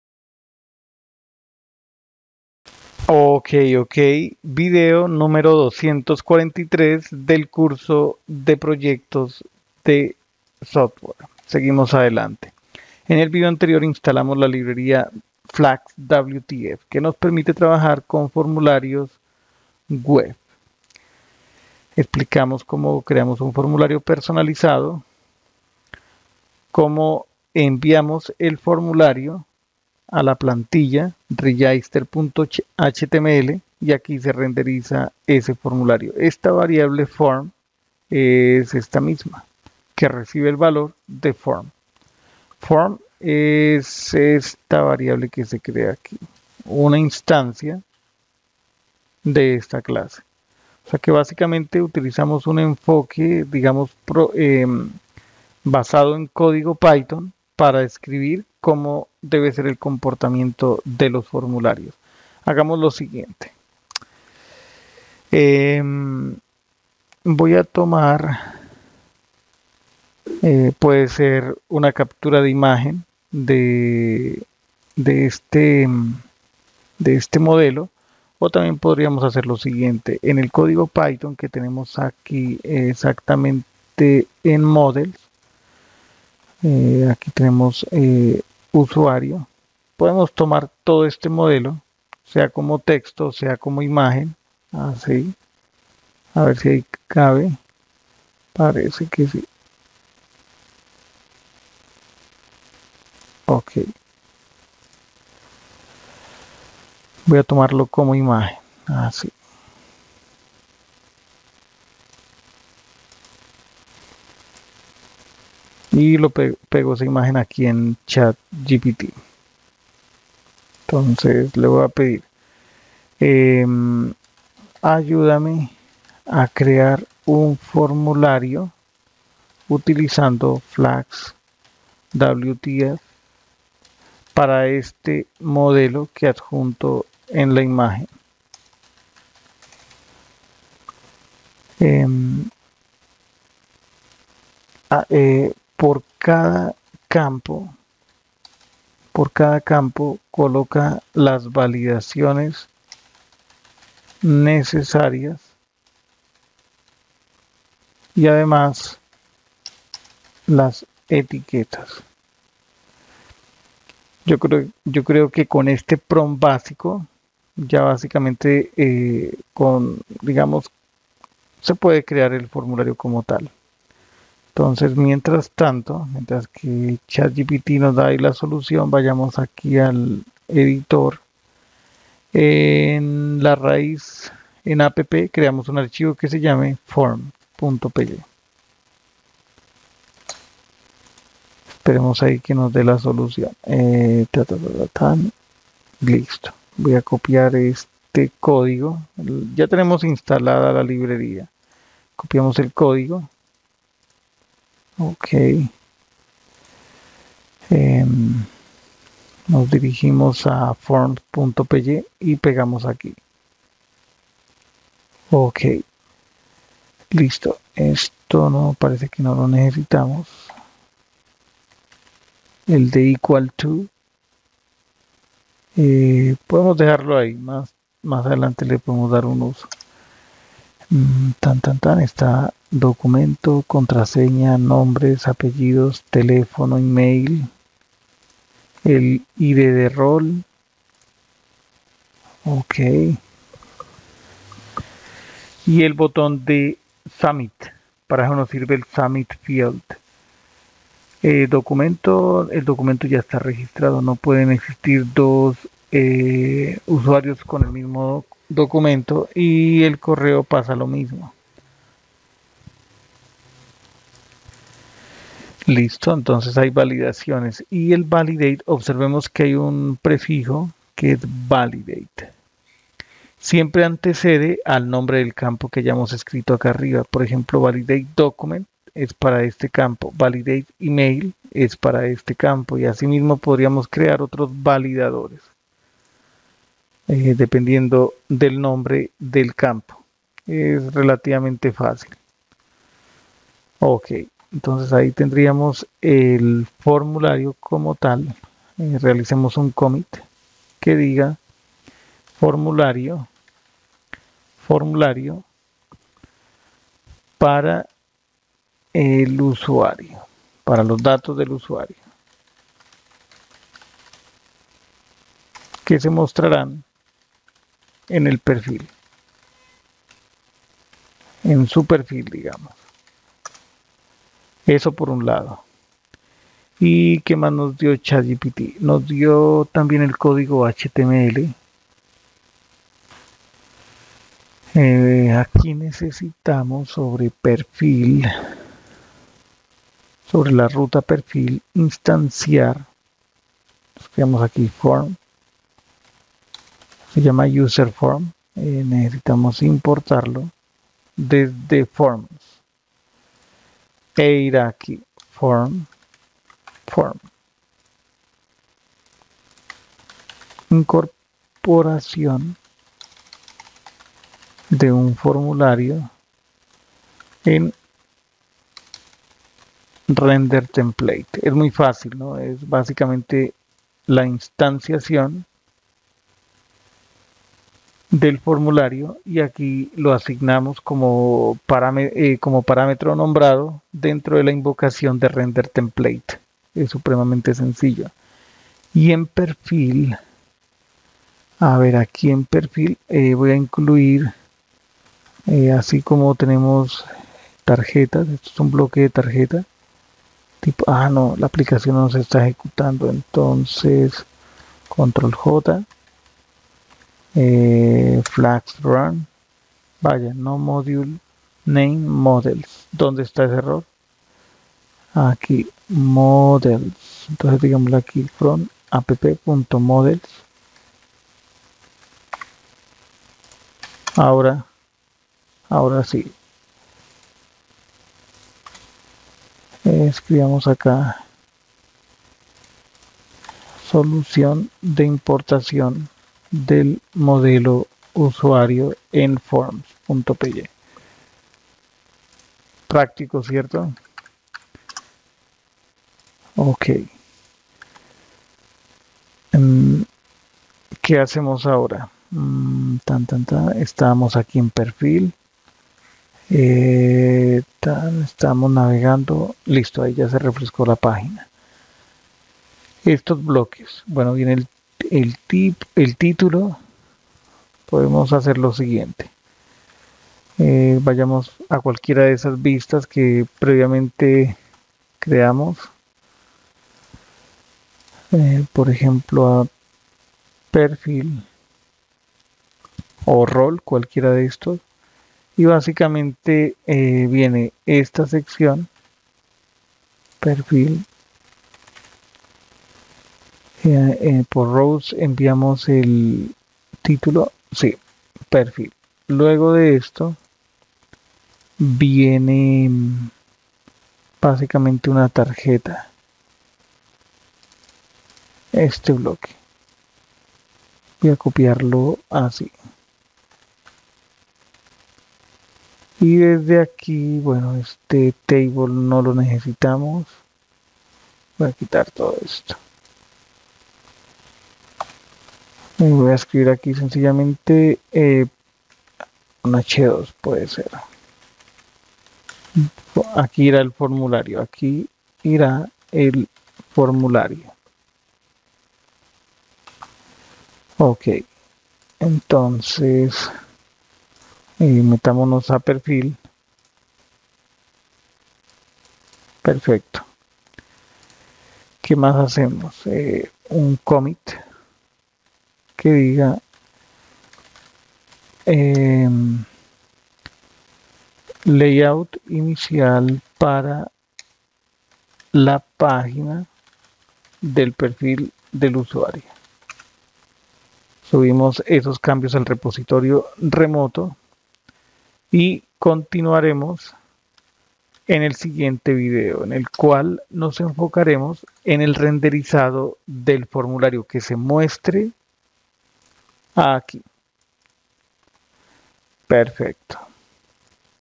243: PROYECTO 7 ChatGPT Nos Ayudó a Crear un Formulario WTForm para los Datos del Perfil del Usuario
243: PROYECTO 7 ChatGPT Nos Ayudó a Crear un Formulario WTForm para los Datos del Perfil del Usuario.